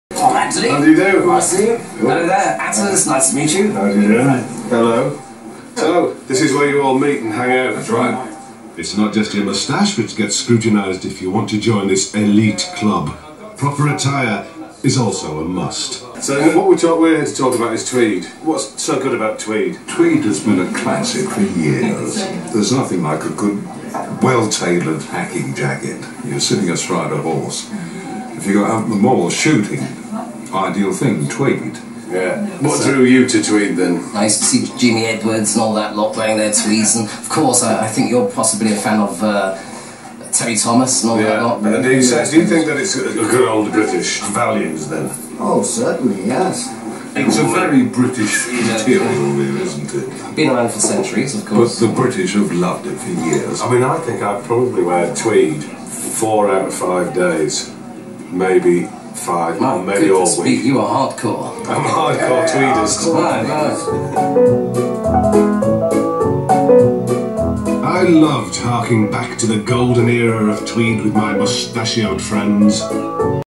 Vic Reeves Meets Atters Attree and Handlebar Club Members - TV Clip
Vic Reeves discusses Tweed with Michael Atters Attree and some other Handlebar Club members.